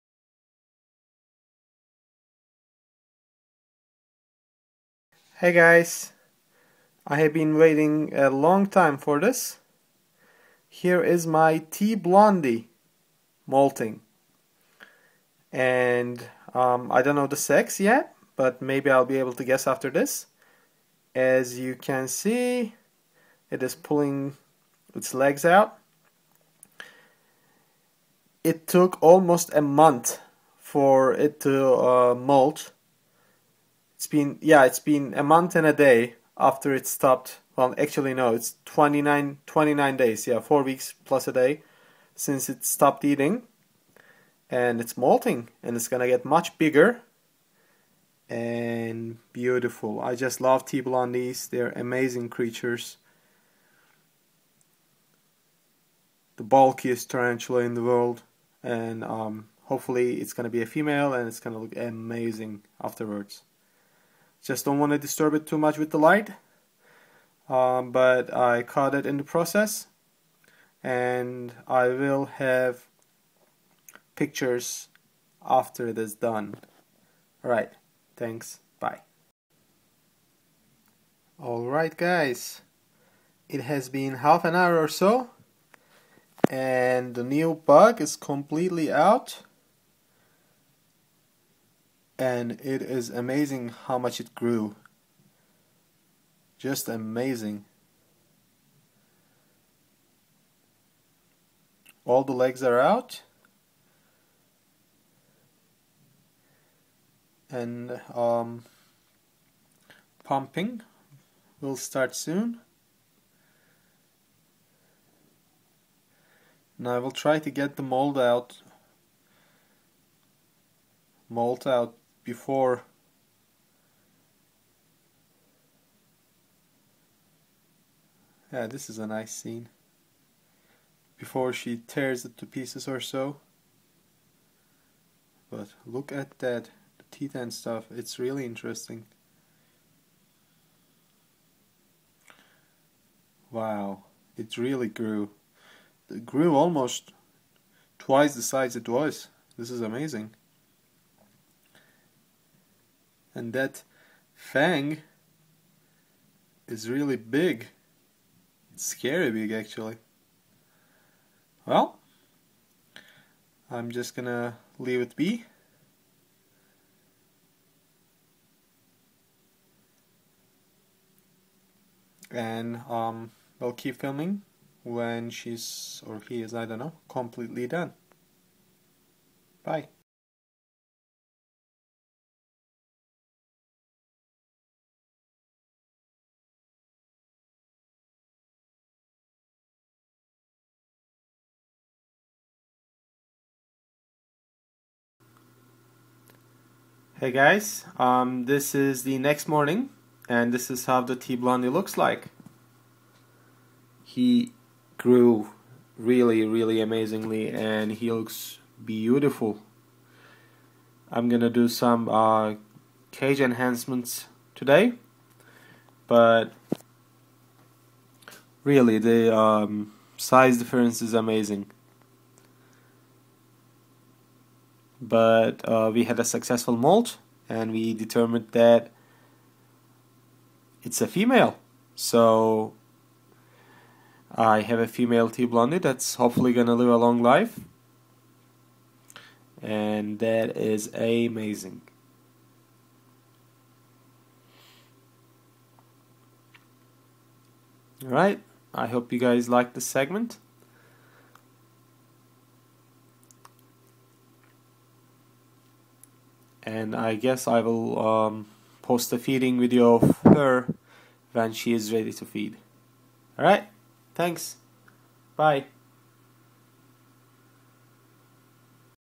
Theraphosa blondi (Goliath Bird Eater) Molting
Here is my Theraphosa blondi (True blondi from French Guiana) molting.

I start with a before picture and continue with some footage from the actual molting process, the size of the exuvium, exuvium close up pictures for sexing and end with the next day footage.

Yasin